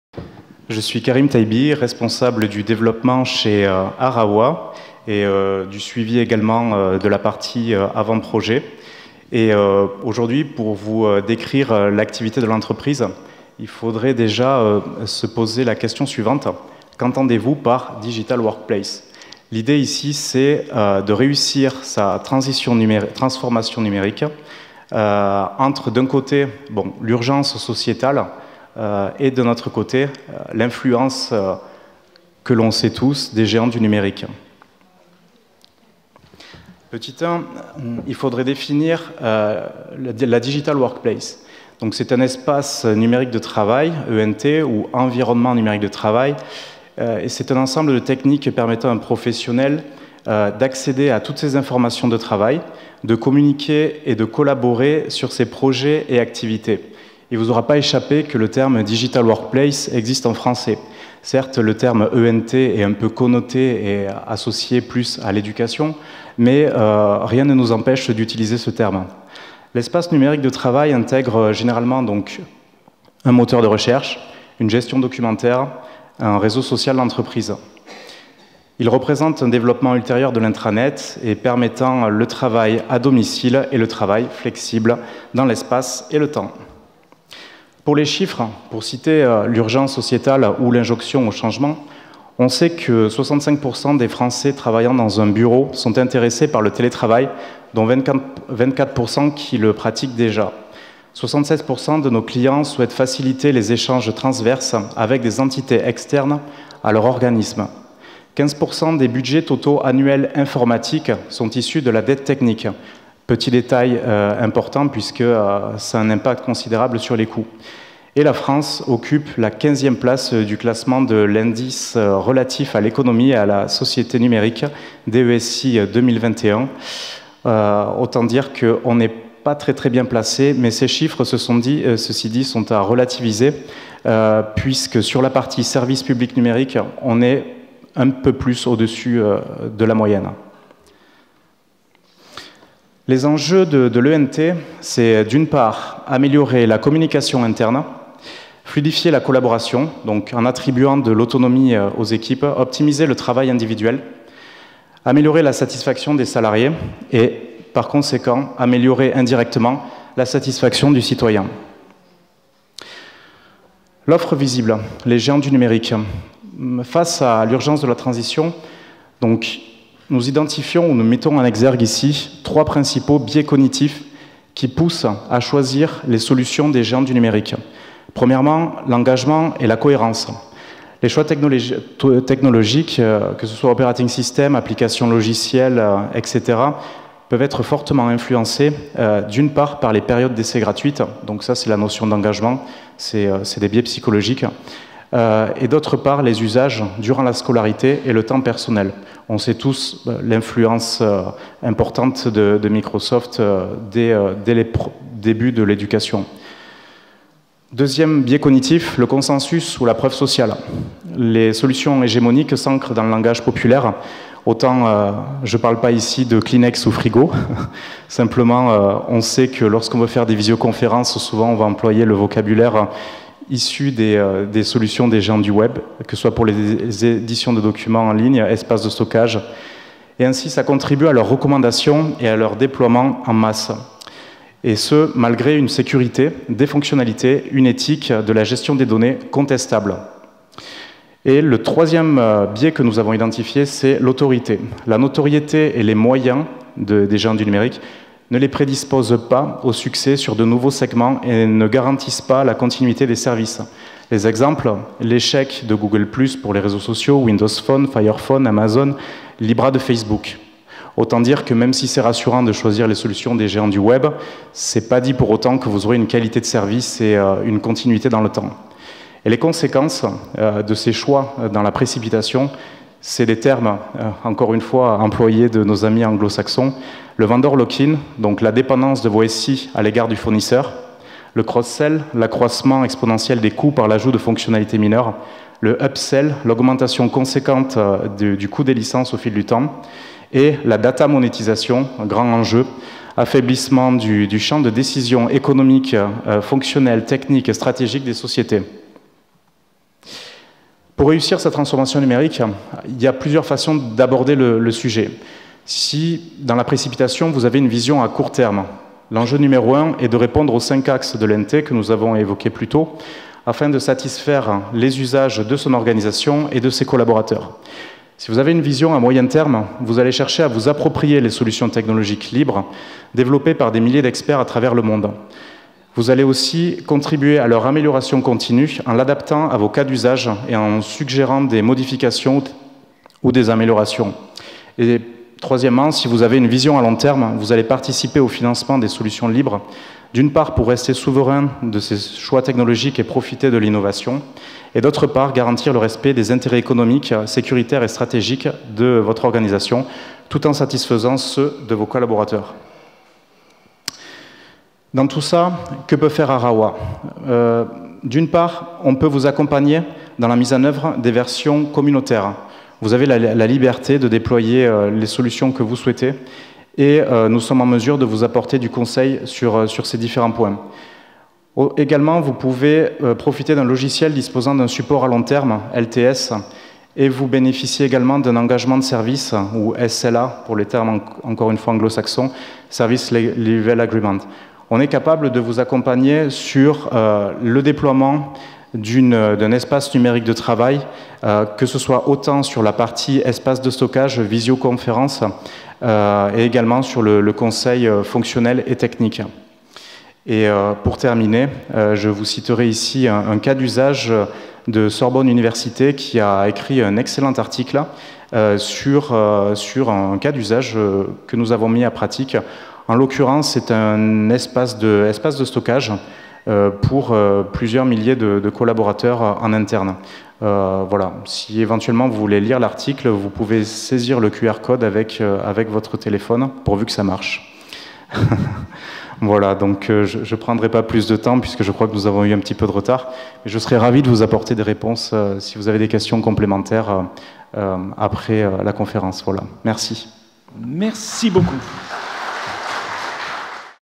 Présentation ARAWA aux 20 ans de l'ADULLACT
La société ARAWA présente son activité lors des 20 ans de l'ADULLACT.